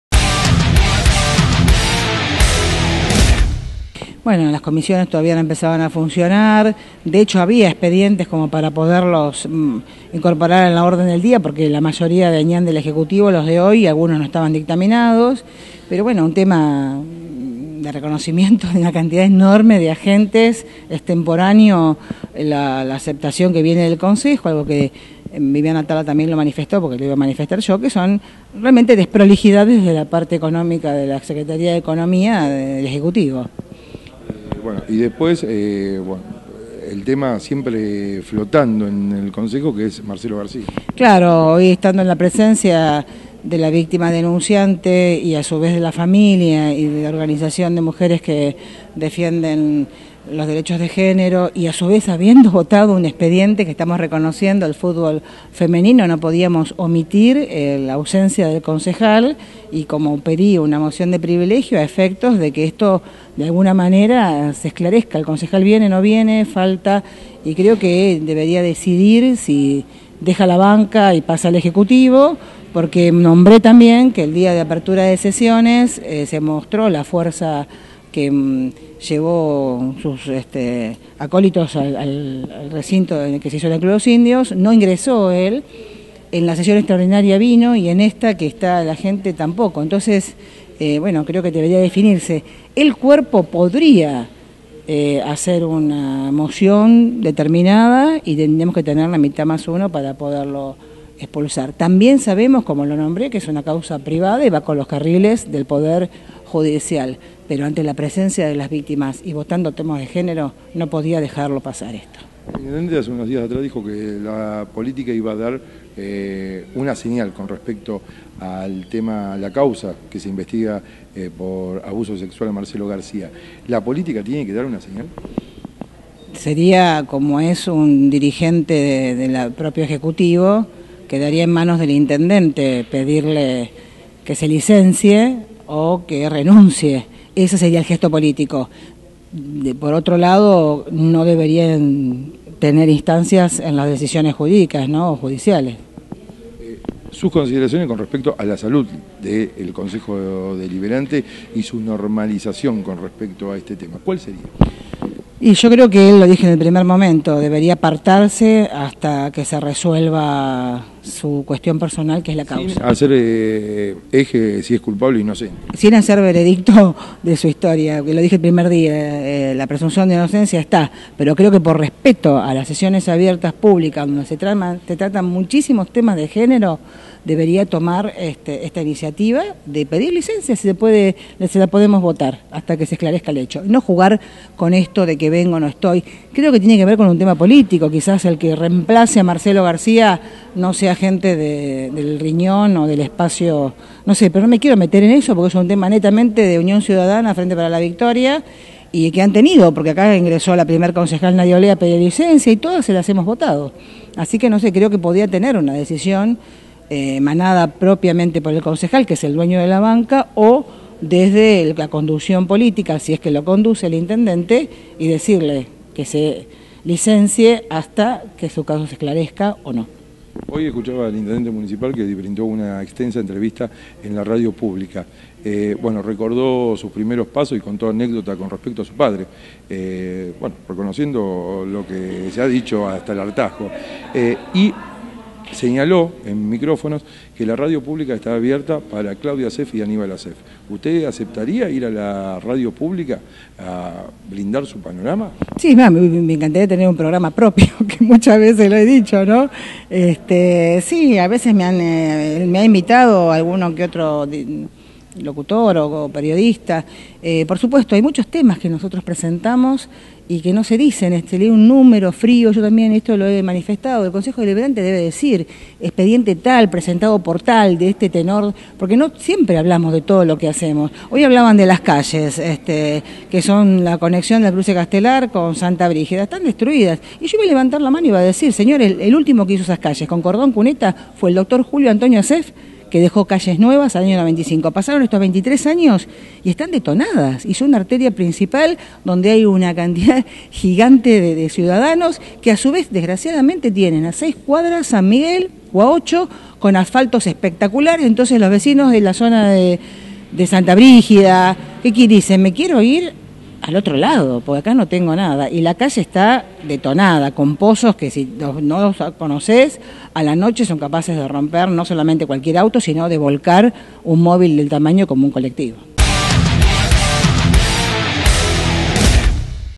Una sesión marcada por la conquista de los derechos de las mujeres y la ausencia del concejal García
Entrevista con la concejal Claudia Asseff (Cambiemos)